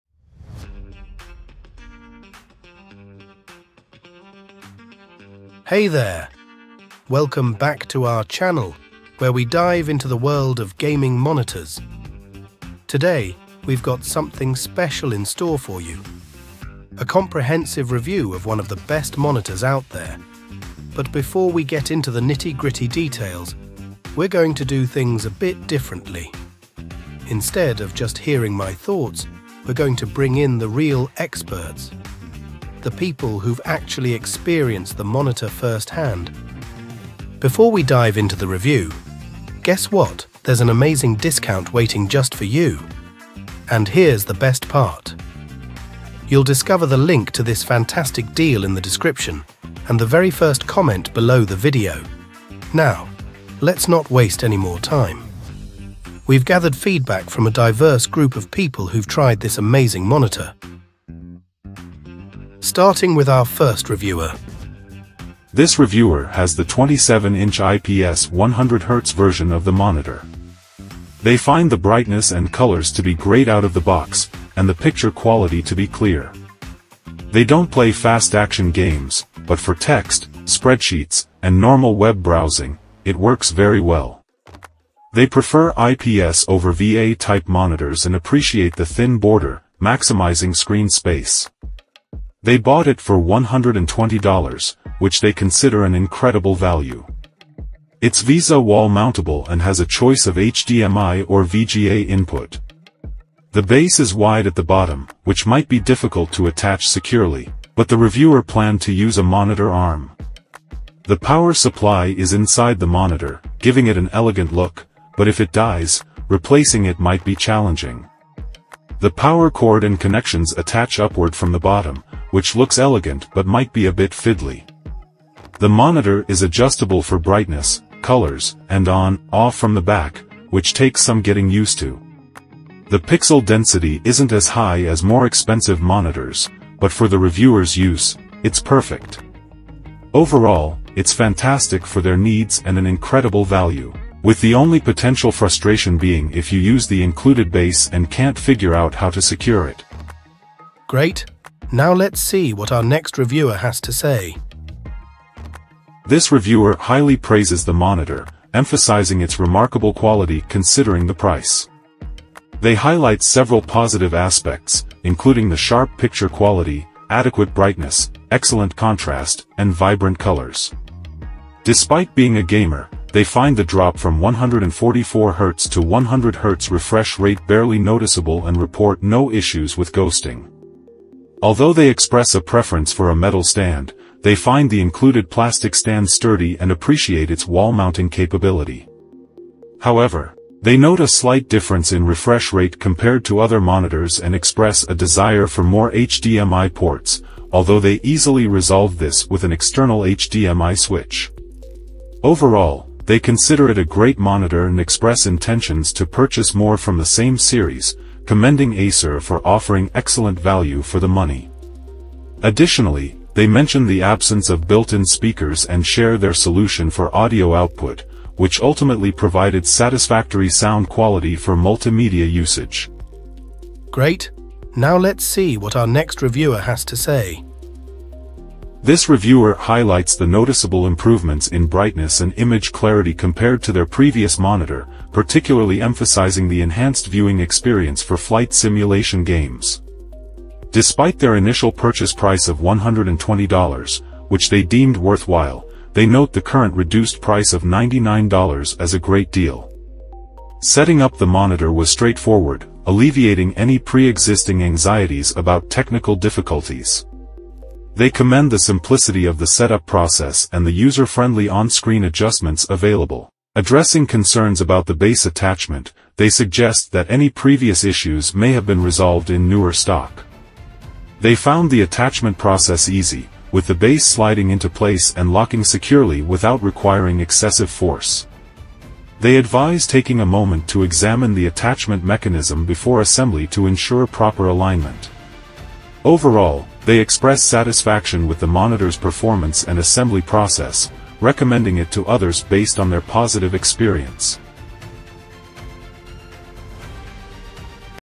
Important Insights into the Acer KB272 Hbi Full HD Gaming Office Monitor : A Review
the link from the video
Experience seamless productivity and immersive gaming with the Acer KB272 Hbi Full HD Gaming Office Monitor in this detailed review. Boasting a 27-inch display, delve into crisp Full HD visuals for work and play. Uncover its ergonomic design and adjustable stand, ensuring comfort during extended use. Explore its fast refresh rate and AMD FreeSync technology, delivering smooth gameplay without screen tearing. With versatile connectivity options, including HDMI and DisplayPort, this monitor caters to diverse setups. Discover its color accuracy and sharpness, enhancing both office tasks and gaming experiences. Whether you're a professional or a gamer, find out if this monitor suits your needs.